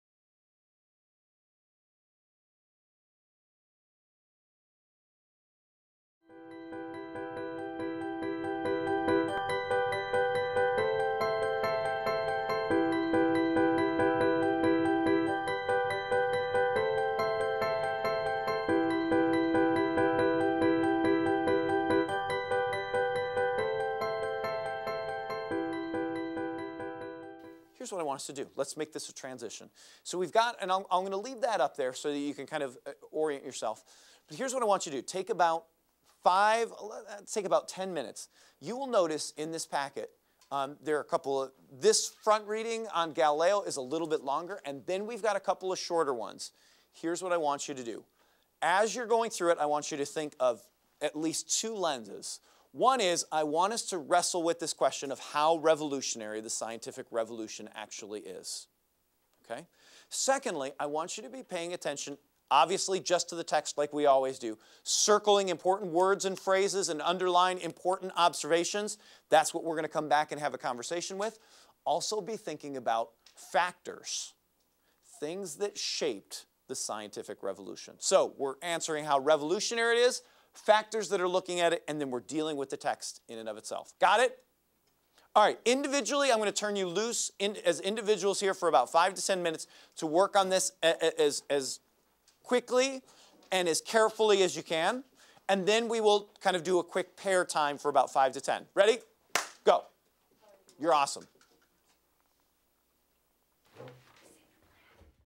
Teacher manages transitions - Example 4
Transitions occur smoothly, with little loss of instructional time due to clear directions.